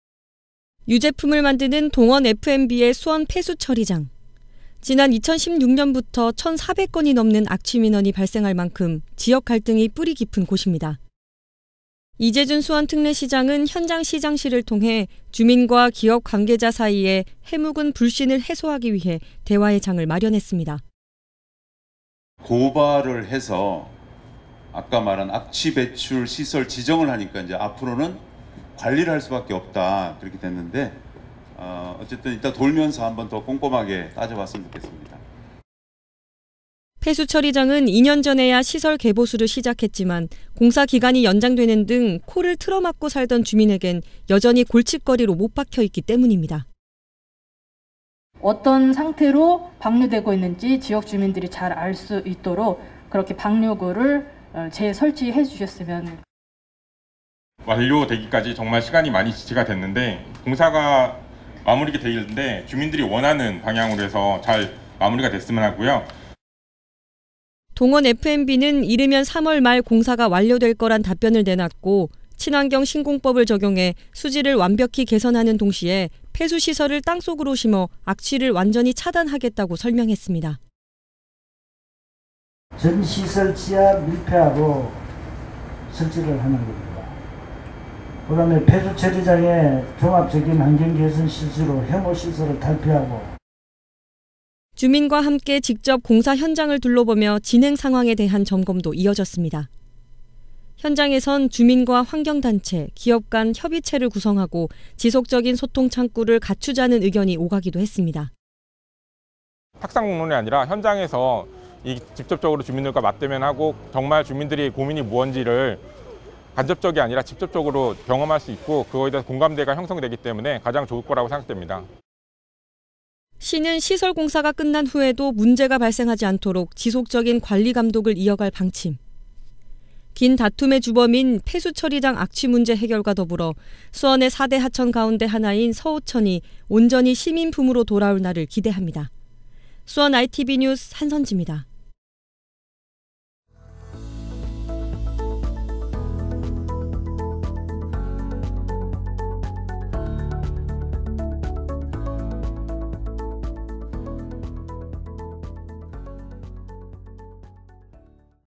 '서호천 골치' 동원F&B 악취... "3월 말 해결"
유제품을 만드는 동원F&B의 수원 폐수처리장.

지난 2016년부터 1천4백 건이 넘는 악취 민원이 발생할 만큼 지역 갈등이 뿌리 깊은 곳입니다.

[싱크] 이재준 수원특례시장
“(수원시가 동원F&B를) 고발을 해서 아까 말한 악취 배출 시설 지정을 하니까 앞으로는 관리를 할 수밖에 없다고 그렇게 됐는데, 이따가 돌면서 한 번 더 꼼꼼하게 따져봤으면 좋겠습니다.”

폐수처리장은 2년 전에야 시설 개보수를 시작했지만, 공사 기간이 연장되는 등 코를 틀어막고 살던 주민에겐 여전히 골칫거리로 못 박혀 있기 때문입니다.

[싱크] 김경이 환경단체 사무국장
“어떤 상태로 (폐수가) 방류되고 있는지 지역주민들이 잘 알 수 있도록 그렇게 방류구를 재설치해주셨으면...”

[싱크] 김대섭 인근 아파트 입주자대표
“완료되기까지 정말 시간이 많이 지체됐는데, 공사가 마무리되면 주민들이 원하는 방향으로 해서 잘 마무리됐으면 하고요.”

동원F&B는 이르면 3월 말 공사가 완료될 거란 답변을 내놨고.

친환경 신공법을 적용해 수질을 완벽히 개선하는 동시에 폐수시설을 땅속으로 심어 악취를 완전히 차단하겠다고 설명했습니다.

[싱크] 공사 책임자

“전 시설 지하 밀폐화로 설치를 하는 겁니다. 그다음에, 폐수처리장에 종합적인 환경개선 실시로 혐오시설을 탈피하고...”

주민과 함께 직접 공사 현장을 둘러보며 진행 상황에 대한 점검도 이어졌습니다.

현장에선 주민과 환경단체, 기업 간 협의체를 구성하고 지속적인 소통 창구를 갖추자는 의견이 오가기도 했습니다.

[인터뷰] 김대섭 인근 아파트 입주자대표
“탁상공론이 아니라 현장에서 직접적으로 주민과 맞대면하고 정말 주민들의 고민이 무엇인지를 간접적이 아니라 직접적으로 경험할 수 있고 그것에 대한 공감대가 형성되기 때문에 가장 좋을 거로 생각합니다.”

시는 시설 공사가 끝난 후에도 문제가 발생하지 않도록 지속적인 관리, 감독을 이어갈 방침.

긴 다툼의 주범인 폐수처리장 악취 문제 해결과 더불어 수원의 4대 하천 가운데 하나인 서호천이 온전히 시민 품으로 돌아올 날을 기대합니다.

수원iTV NEWS 한선지입니다.

동원F&B 수원공장 폐수처리장 / 서호천 인근
주민-기업 대화의 장 마련 / 찾아가는 현장시장실(지난 2월28일)
9년간 민원 지속..주민 “공사 연장 등 불신”
동원F&B “3월 말 공사 완료..악취 차단”